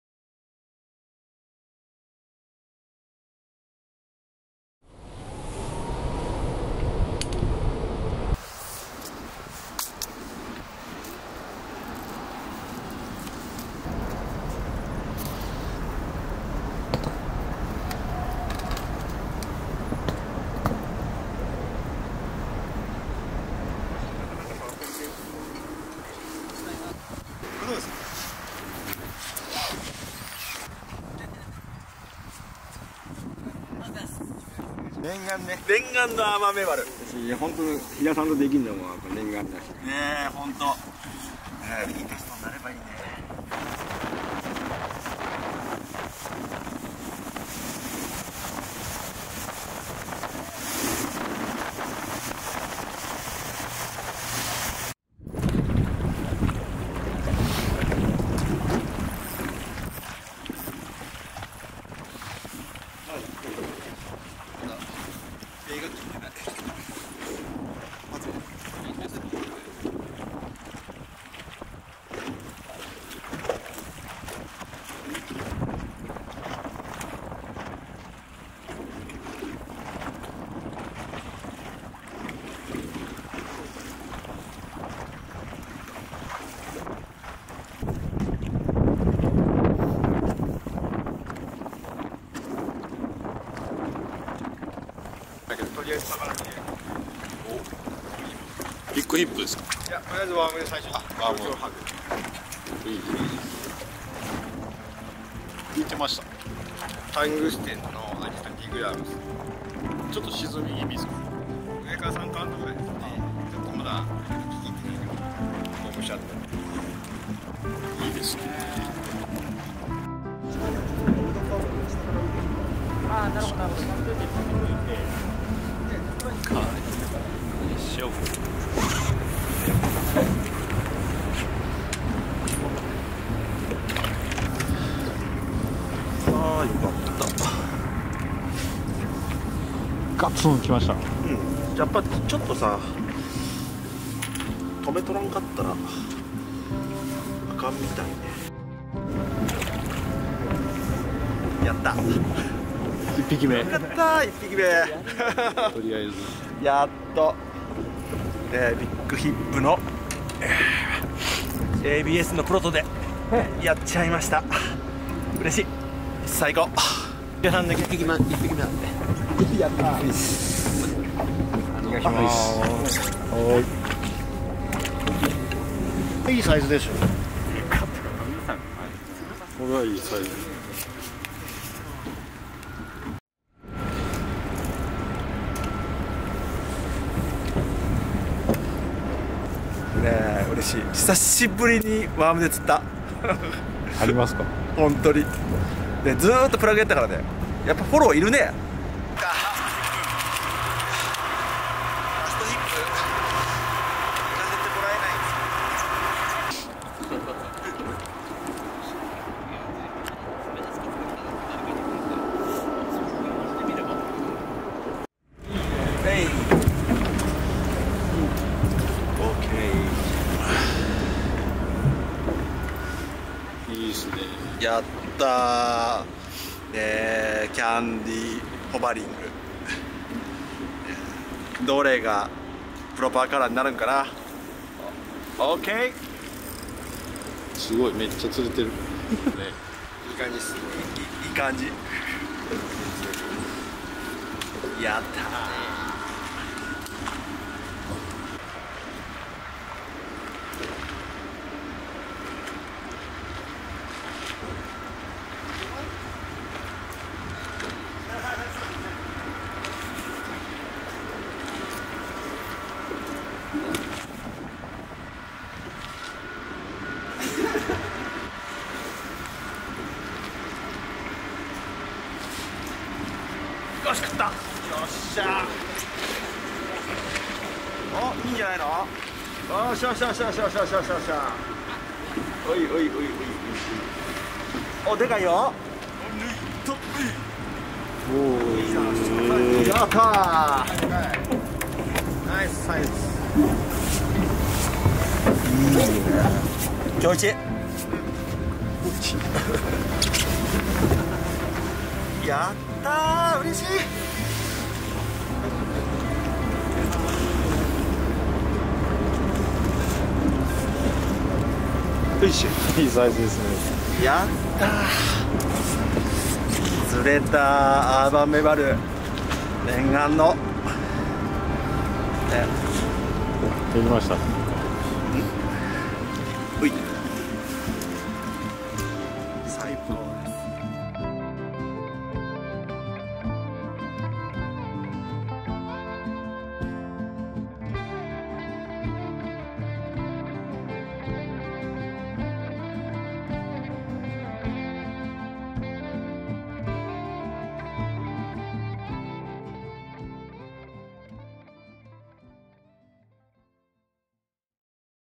BigHipプロジェクトcase.YOKOHAMA
北陸富山発！「みんなで作るメバルルアー」をコンセプトに飛田俊一郎氏が生み出した3Dプリンタープラグ「ビッグヒップ」。全国にたくさん届けたいという想いからABS量産型をプロデュースすべく2021年12月にスタートしたのがTICT×ビッグヒップ プロジェクト。
2023年1月に横浜から出船した東京湾アーバンメバルでのテスト風景。
Supported by icfc アイランドクルーズ